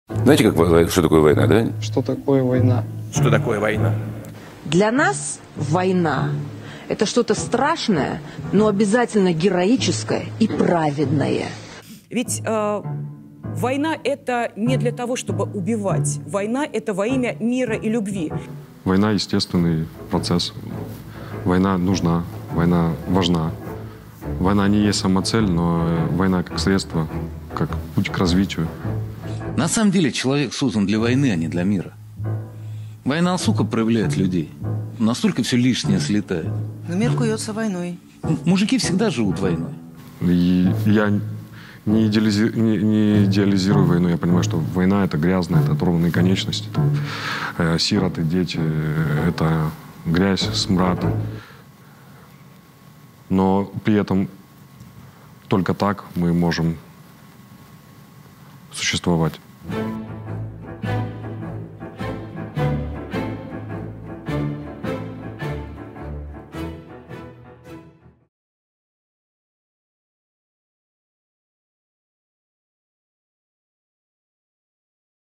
Как оправдывают войну российские пропагандисты
«Война — во имя мира и любви» и другие любопытные оправдания преступлений армии РФ от бесогонов с российского телевидения.